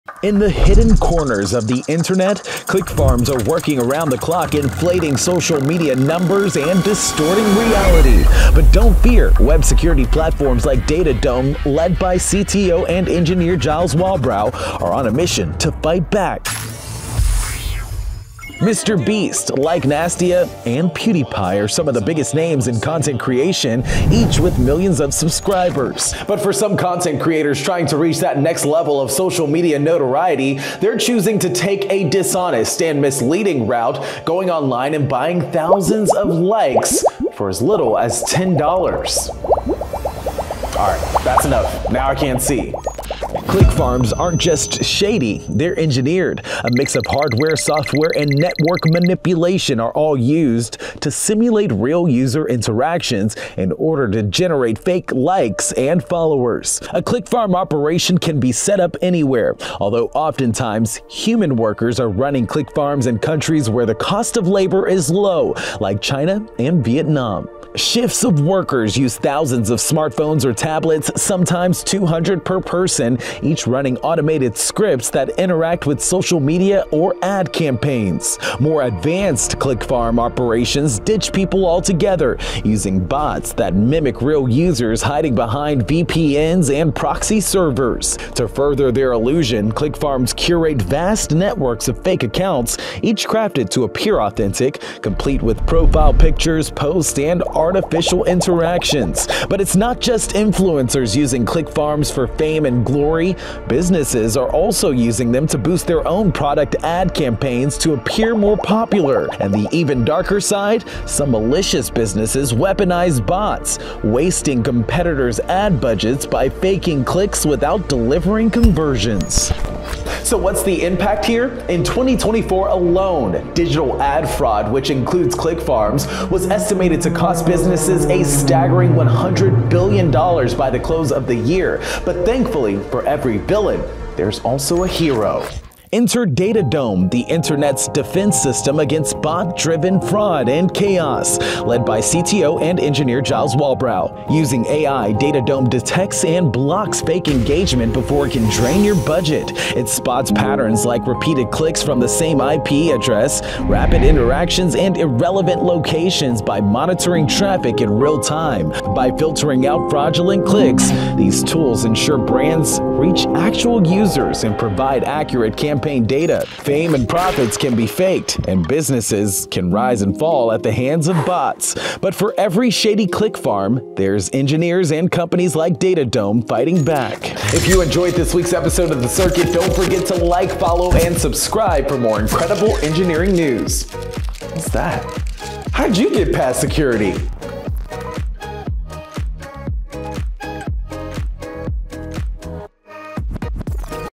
Click Farms Create Fake Likes and Subscribers. SEE HOW!
How far are you willing to go for internet fame?  Learn more about the engineering of click farms from hardware and software to network manipulation!

Whether you’re interested in social media, artificial intelligence, or engineering, you don't want to miss this exciting episode!

VIDEOS
Premiers pas et performance chez DATADOME, avec Gilles Walbrou courtesy of WeLoveDevs